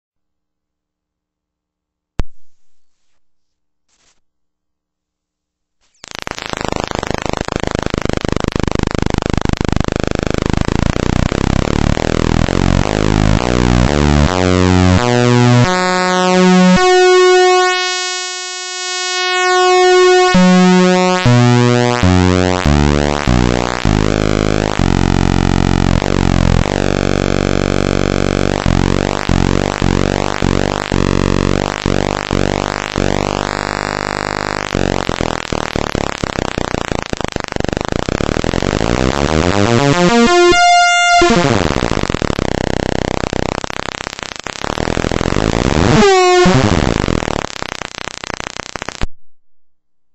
Atari Punk Console squarewave synth !
Atari Punk Console squarewave synth ! Better known as the stepped tone generator from Forest Mims project books. This uses a 556 timer chip and some potentiometers, mounted in a miniature Atari console from a broken game system.